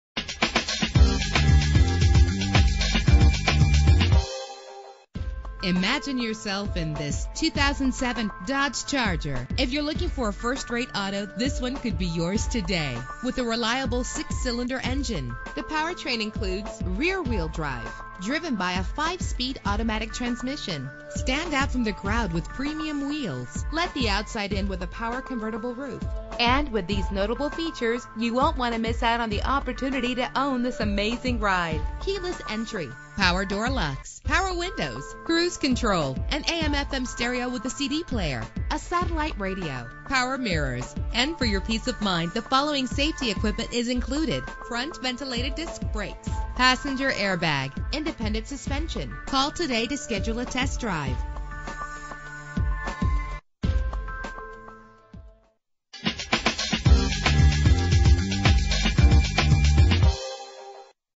2007 Dodge Charger - Little Rock AR
Year: 2007
Make: Dodge
Model: Charger
Trim: Base
Engine: 3.5 liter 6 cylinder 24 valve
Transmission: 5-Speed Automatic
Color: Blue
Mileage: 67952
Address:
4600 South University Ave